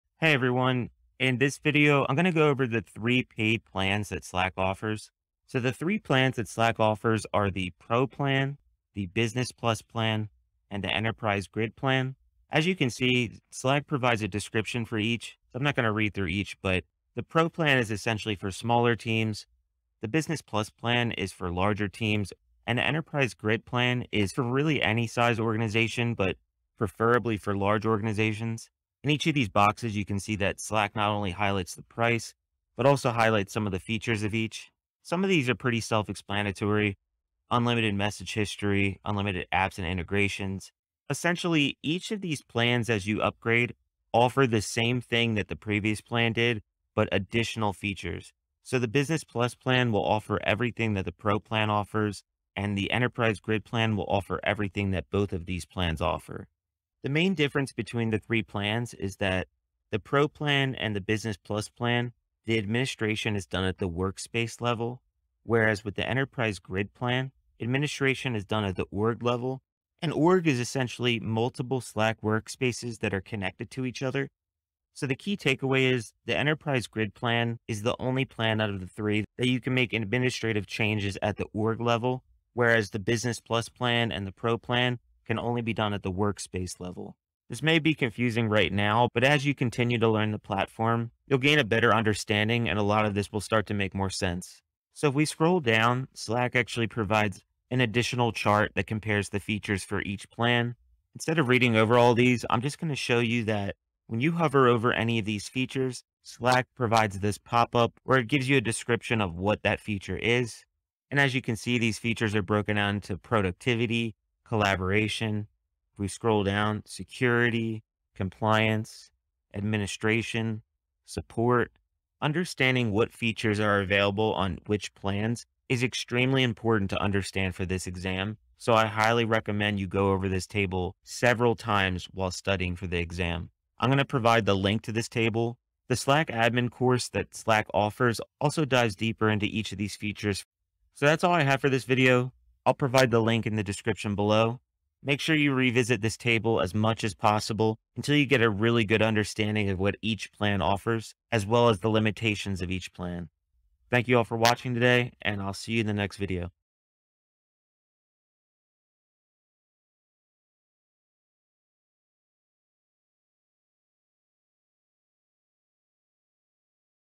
Slack Admin Mini-Course: Slack Paid Plans
In this video, I go over the three paid plans that Slack offers. Understanding the differences between the capabilities and limitations of each plan is extremely important when preparing for the Slack Admin exam.